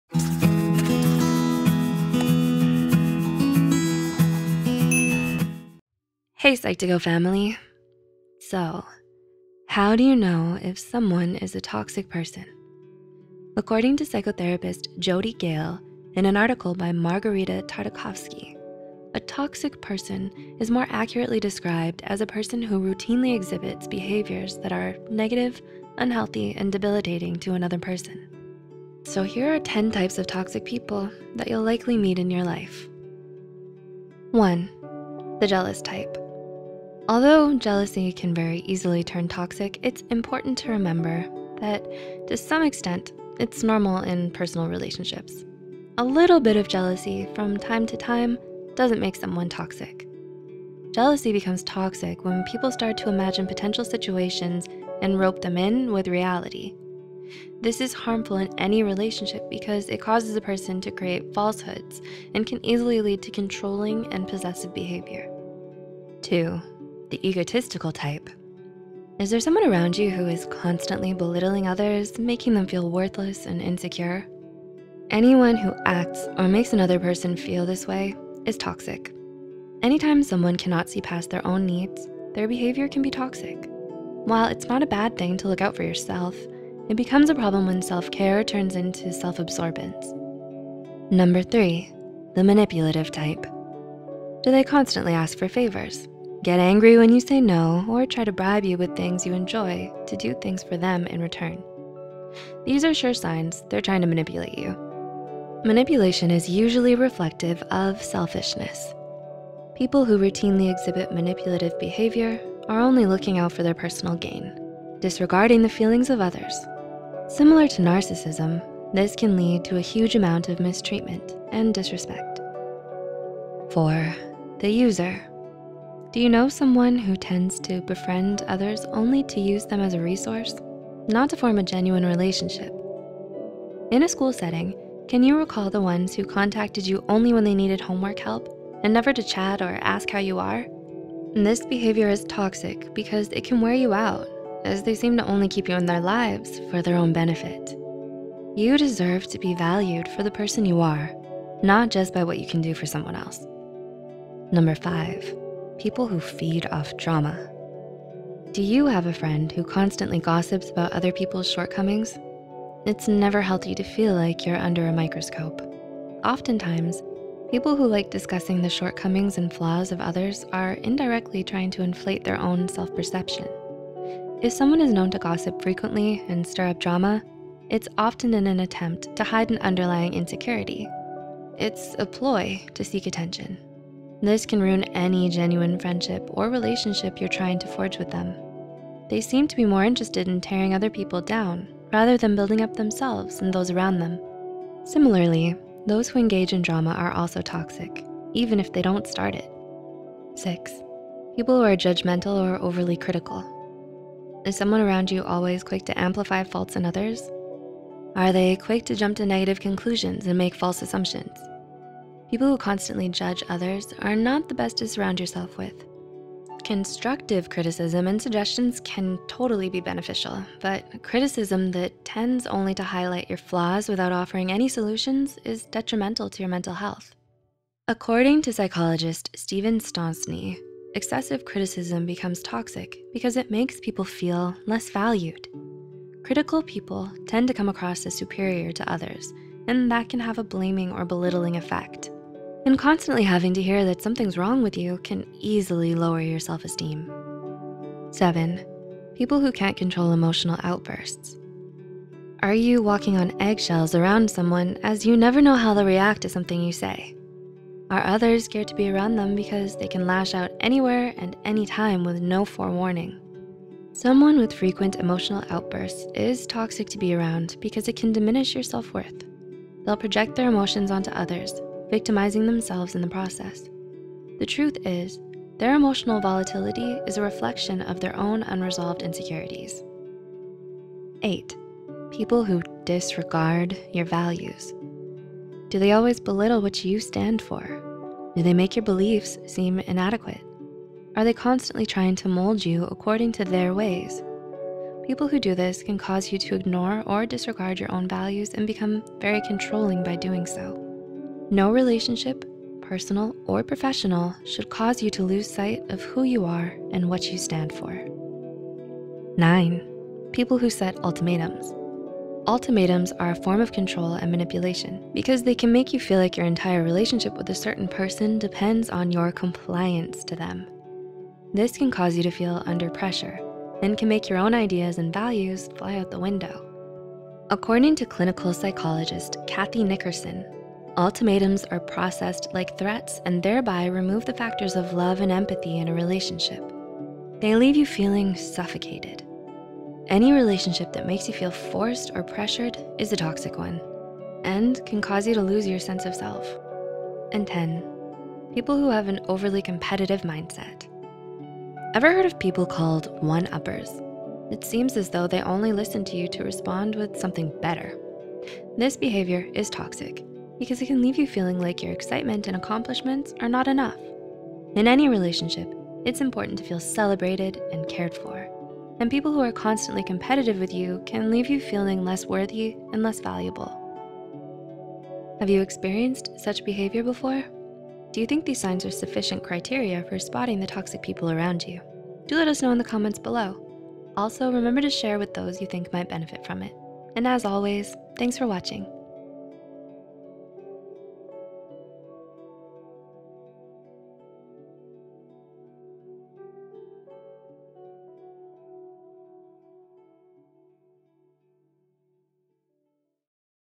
10 Toxic People To Avoid
Toxic people are more accurately described as people who routinely exhibit behaviors that are negative, unhealthy, and debilitating to others. Do you have a toxic person in your life? While these behaviors are toxic, it is important to recognize that the person may be fighting emotional battles of their own. However, we should still be more aware of people like this because they can have a huge impact on your performance and happiness.

If some of these types described you and you're feeling guilty of engaging in some of these negative behaviors yourself, but you're still unsure if you're really becoming toxic, we've also made a video on the signs you've become a toxic person

Writer: Ria Parikh
Script Editor: Rida Batool
Script Manager: Kelly Soong
VO: Amanda Silvera
YouTube Manager: Cindy Cheong

References
Bayes-Fleming, Nicole. “The Mind-Traps that Lead to Toxic Jealousy”. Mindful. 14 December 2018.

Imafidon, Casey. “10 Types of Toxic People You Should Be Careful Of”. Lifehack. N.d.

Lancer, Darlene. “How Insecurity Leads to Envy, Jealousy, and Shame”. Psych Central. 8 October 2018.

Nazareth, Felicia. “10 Types Of Toxic People To Avoid In Life & Toxic Personality Traits”. Align Thoughts. 18 December 2017.

Perron, Josée. “5 Types of Toxic People Who Will Poison your Path if you Let Them”. You Choose The Way. 26 June 2018.

Stosny, Steven. “What’s Wrong with Criticism”. Psychology Today. 18 April 2014.

Tartakovsky, Margarita. “What’s a Toxic Person & How Do You Deal With One?”. Psych Central. 8 July 2018.

Tartakovsky, Margarita. “Why Ultimatums Are Actually Destructive to Your Relationship”. Psych Central. 8 July 2018.

Do you have any personal stories or video requests that you would like to share with us?